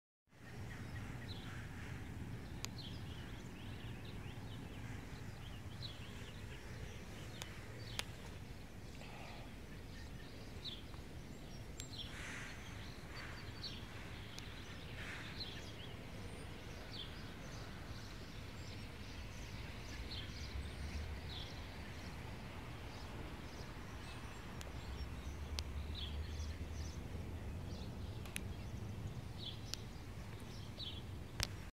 chembusting with no pipe, before
I set this up at 930 am. I made this to show how although pipes have way more advantages. I just want to point out the sheer power of a orgone pyramid made to the phi ratio and aligned to magnetic north. Let's not forget that star clusters are said to clear blocked ley lines and aid geopathic stress, in the area where they are located. I have come to like the nubian shape more than the giza shape. The difference is that the giza shape utilizes the phi ratio in the base measurements, whereas the nubian uses phi in the uprights. The nubian shape is said to be something like 200 times stronger of an output of energy. From all i have personally seen with my own two eyes, they are stronger for sure.